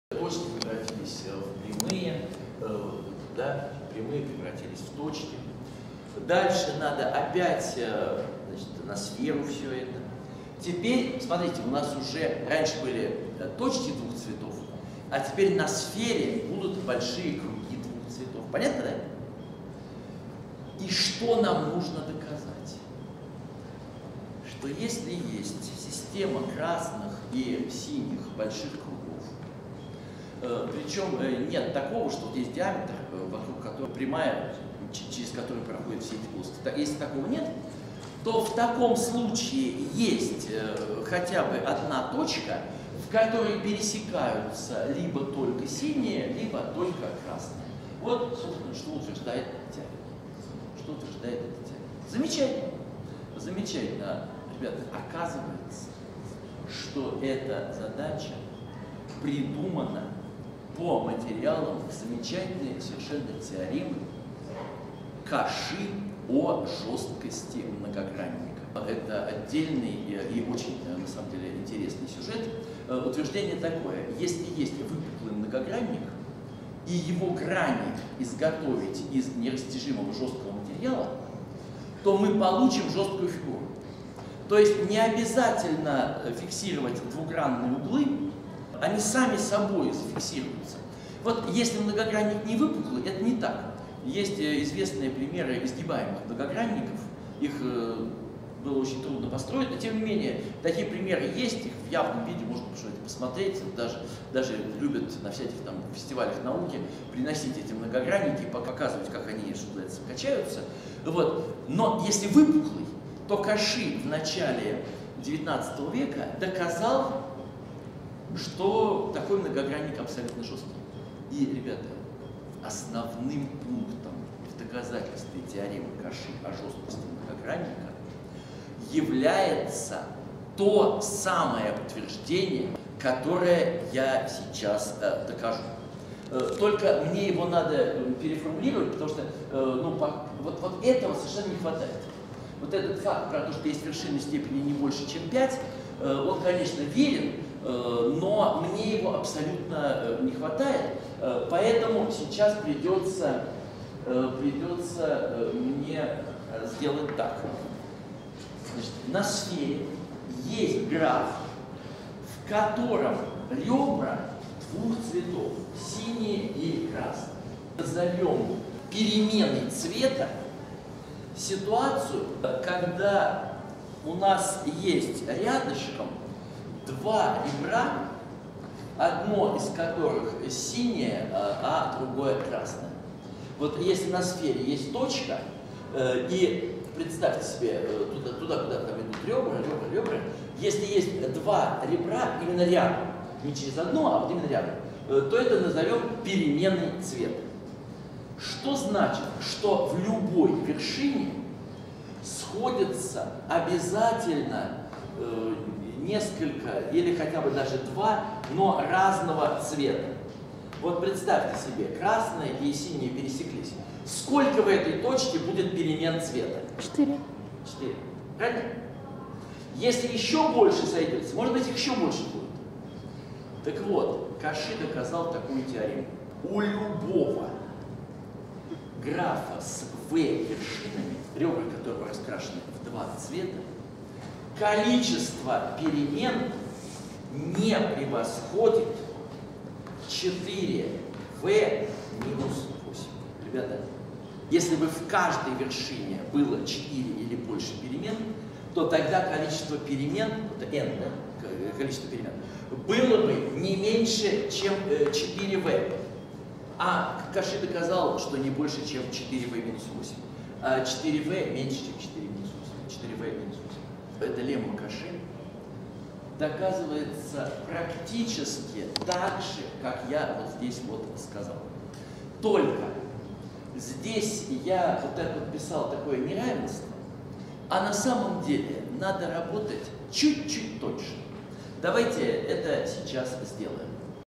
11 Рёбра двух цветов и лемма Коши о переменах цвета
Для любых двух конечных не пересекающих друг друга и не лежащих ни на какой прямой множеств точек существует прямая, пересекающая одно из этих множеств, причём более чем по одной точке.
Двойственное утверждение (для сферы): для любых двух конечных непересекающихся множеств больших кругов, не имеющих общего для всех диаметра, есть хотя бы один диаметр, лежащий более чем на одном из кругов, причём все такие круги лежат в одном из двух множеств.